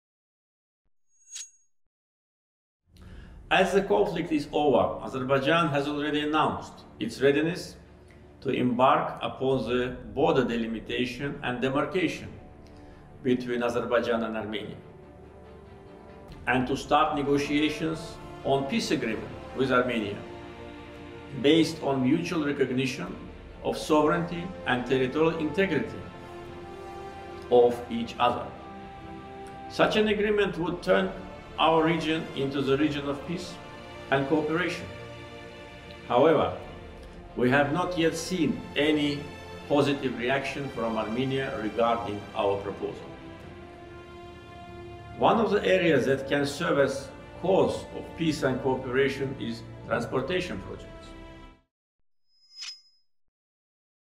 Одной из сфер, служащих миру и сотрудничеству, могут стать транспортные проекты
Ильхам Алиев: “ Одной из сфер, служащих миру и сотрудничеству, могут стать транспортные проекты. В этом контексте, Зангезурский коридор, который соединит основную часть Азербайджана с Нахчыванской Автономной Республикой и Турцией, создаст новые возможности для нашего региона”.